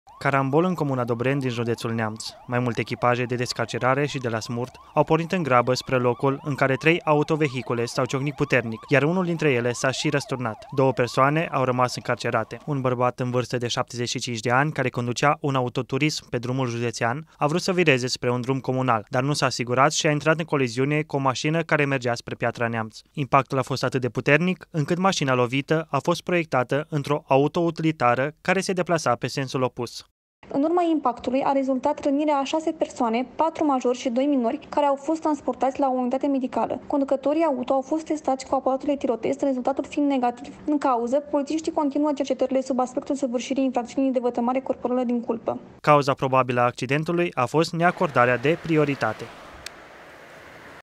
Grav accident în Dobreni
Un accident foarte grav s-a produs în apropierea orașului Târgu Neamț, în localitatea Dobreni. Trei autovehicule au intrat în coliziune, iar una dintre mașini s-a răsturnat. Două persoane au rămas încarcerate. Vă avertizăm, urmează imagini și detalii cu impact emoțional.